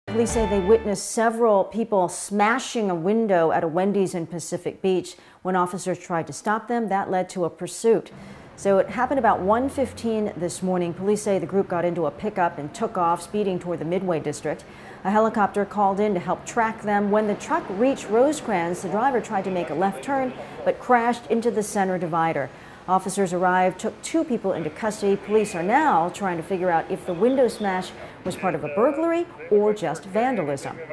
2 arrested after pursuit ends in crash in Midway District
Two people spotted by police smashing a window at a Pacific Beach Wendy’s restaurant were arrested after leading officers on a chase.